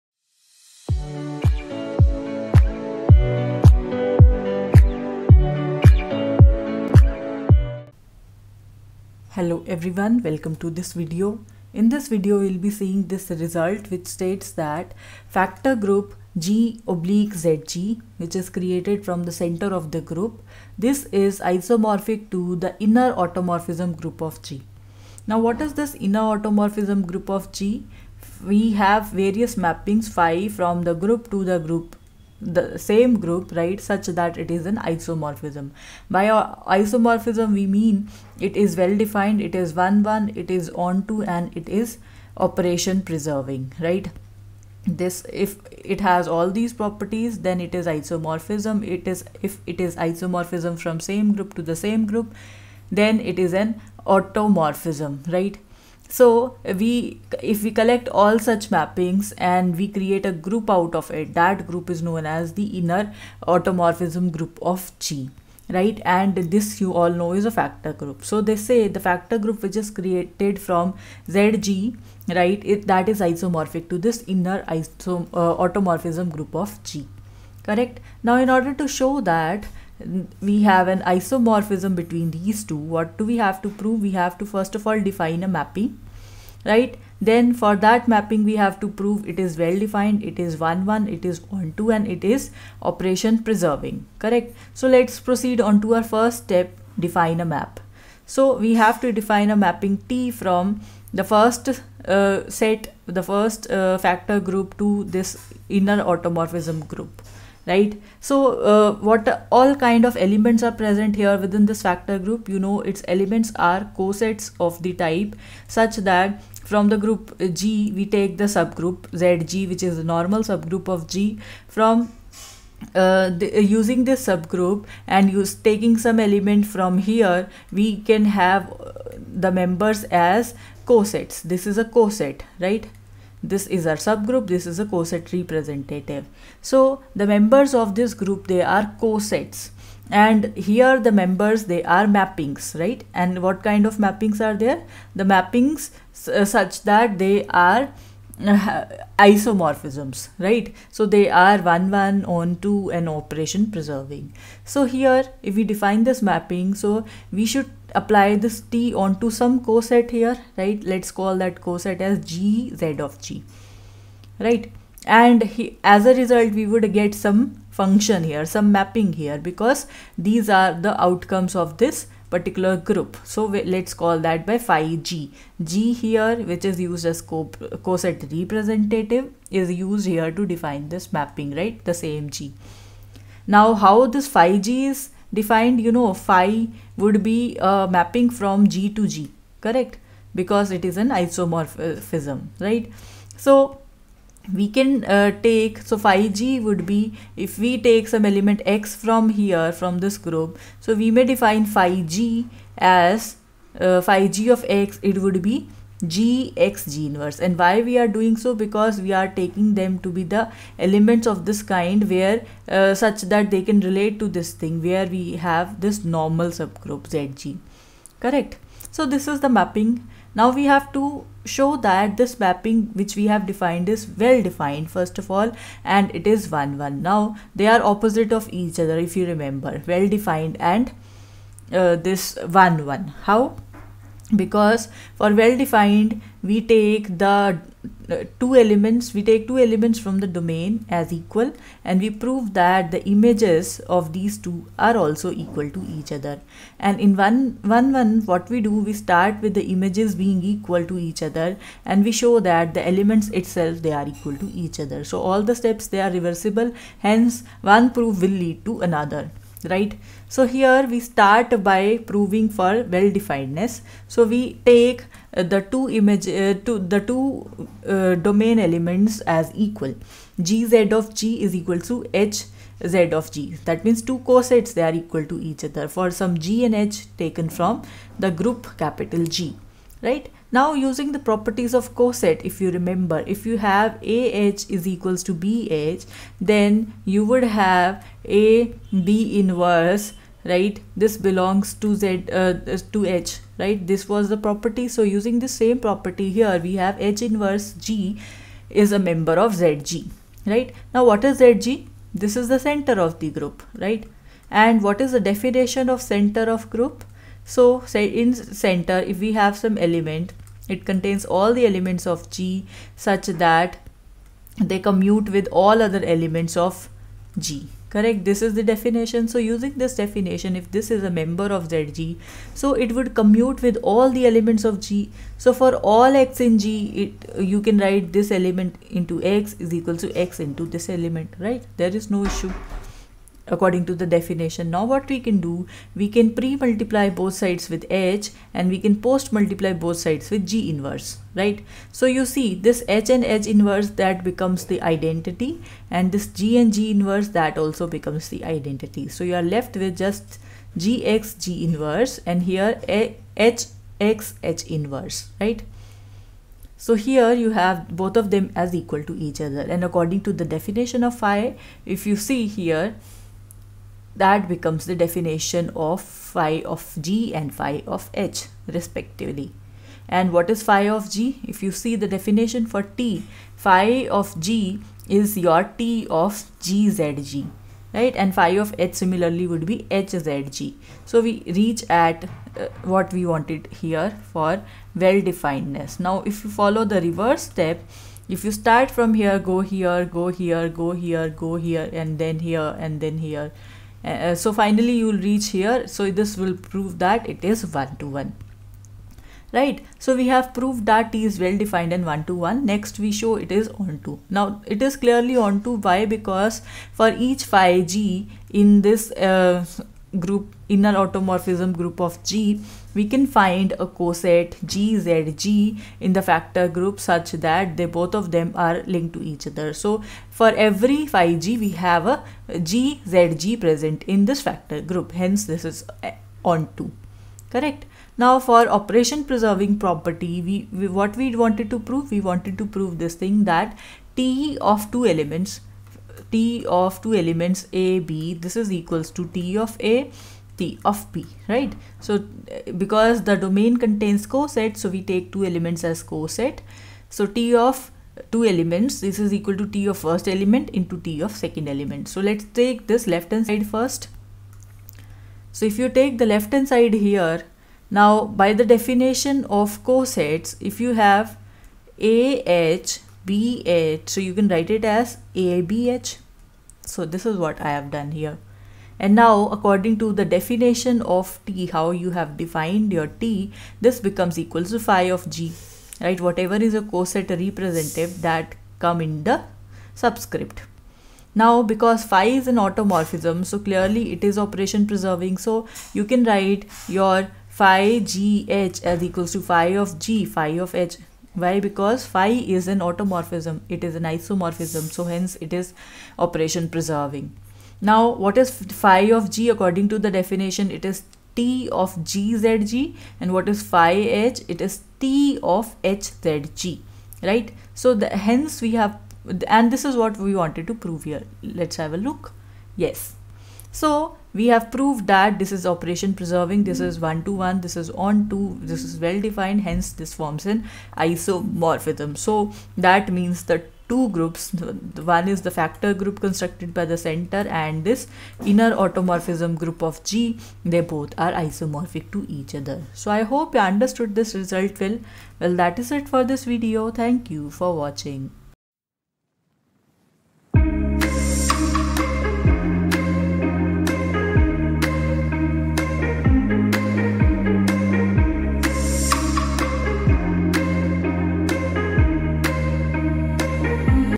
#165: Factor group, G/Z(G) is isomorphic to inner automorphism group of G
This course is based on the group theory portion of Abstract Algebra. Some people call this subject to be a part of modern algebra. To understand the contents, basic knowledge of linear algebra is required.

Happy learning!